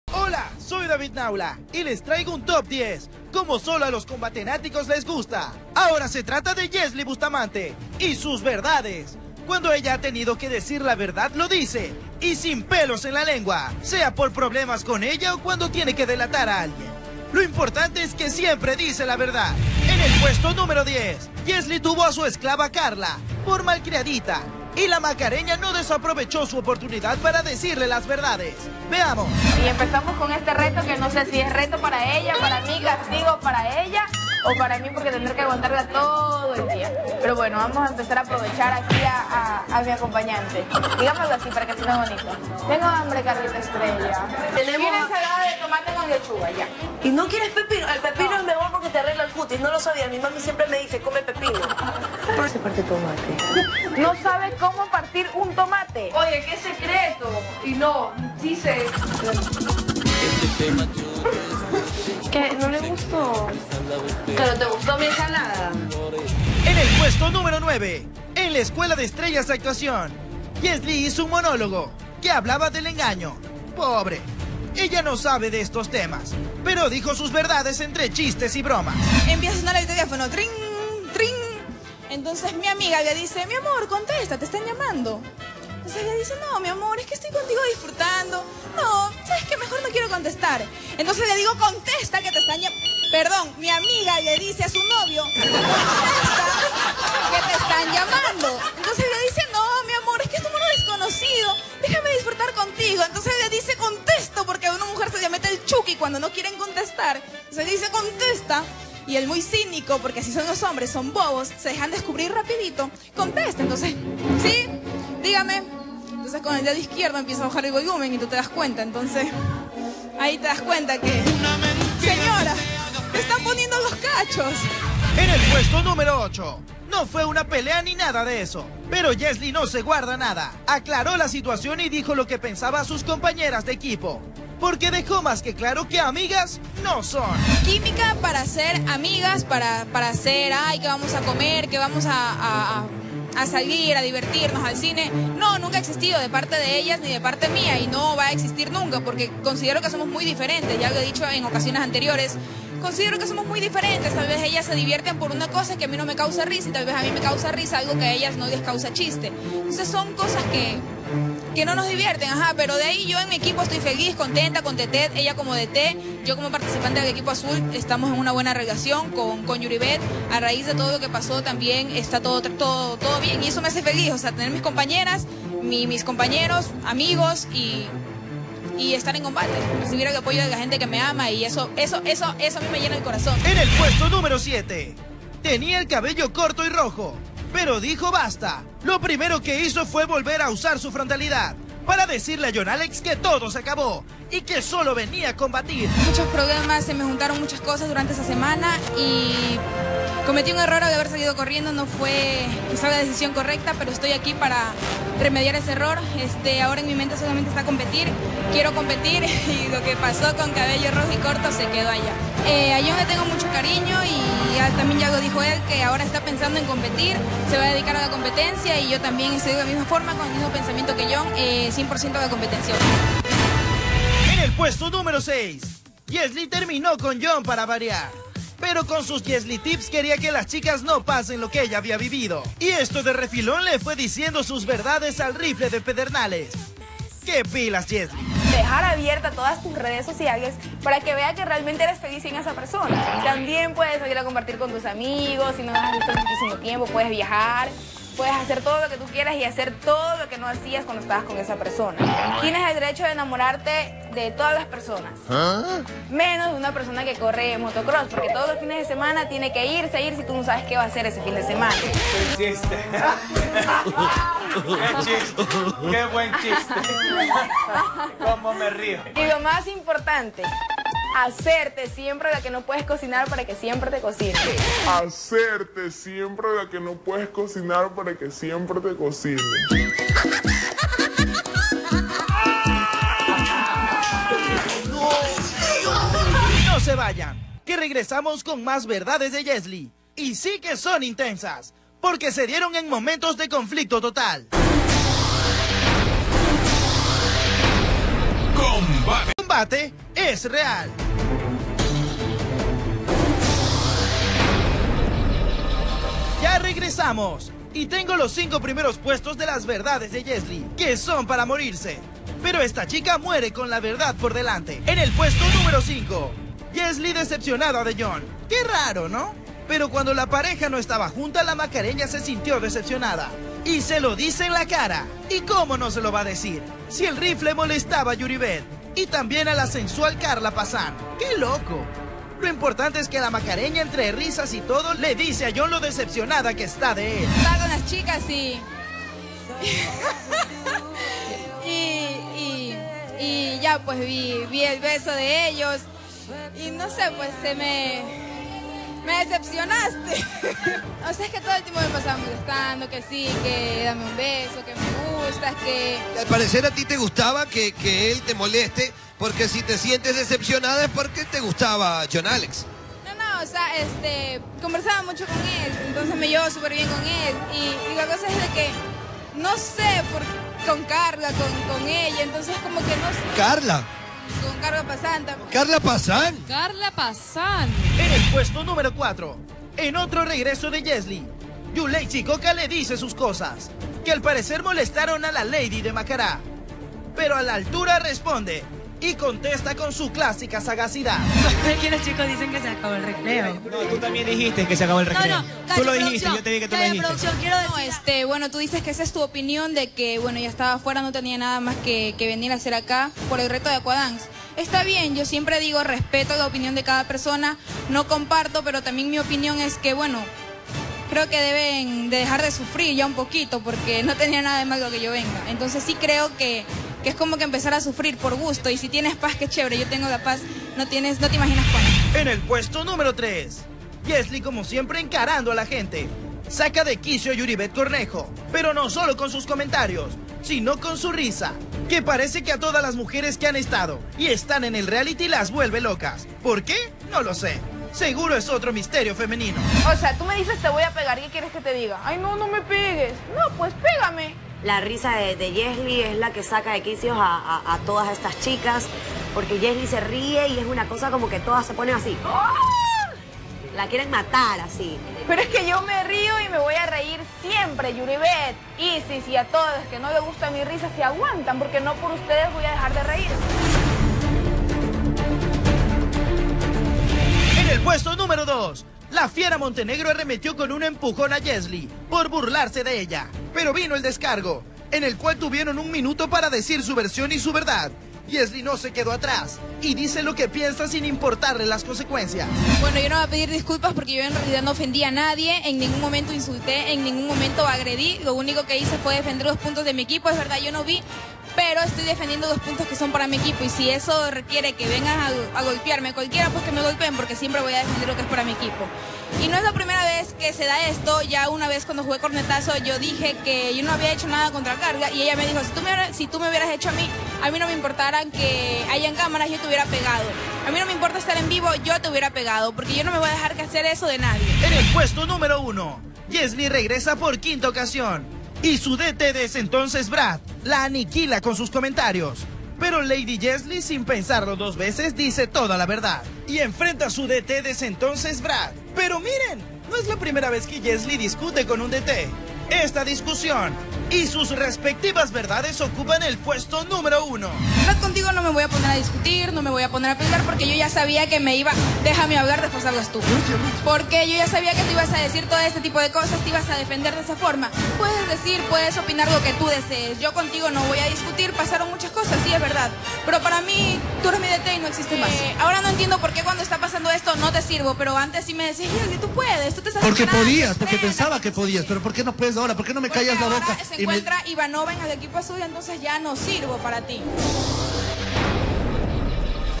Top 10 verdades de Yesly
Yesly es una de las combatientes más polémicas, y es que constantemente ella dice la verdad "sin pelos en la lengua", pese a las consecuencias que esto tenga.

En diversas ocasiones ha emitido sus opiniones sin miedo